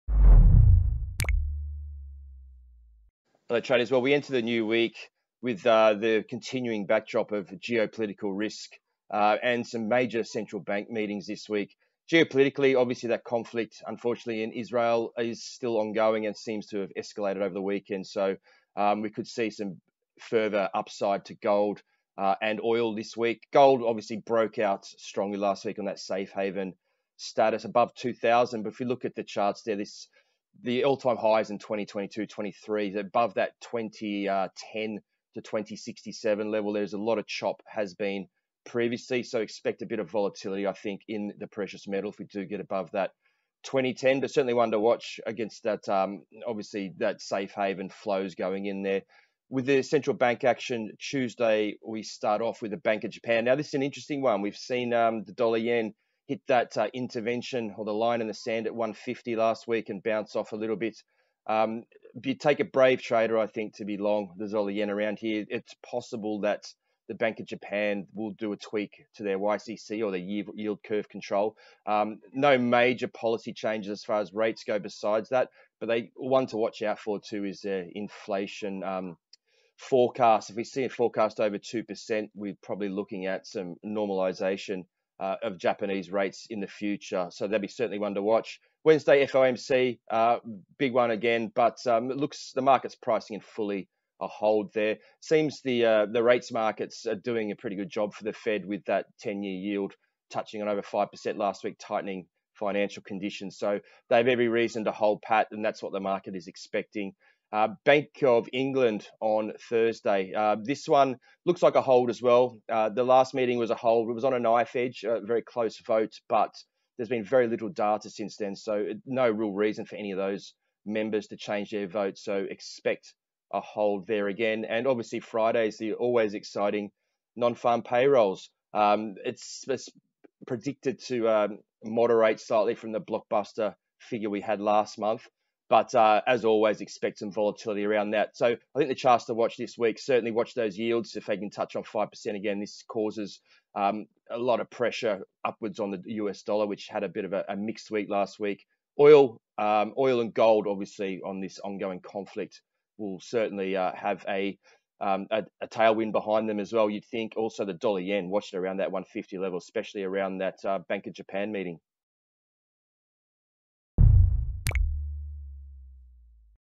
The Week ahead - FOMC, BoE, BoJ and NFP headline
Geopolitics and Central bank meetings look to dominate the market narrative this week. Gold and Oil look to continue their rallies on conflict escalation, JPY at a tipping point ahead of the BoJ policy meeting.

GO Markets Head of Research gives his take on the fundamental and technical levels to watch for.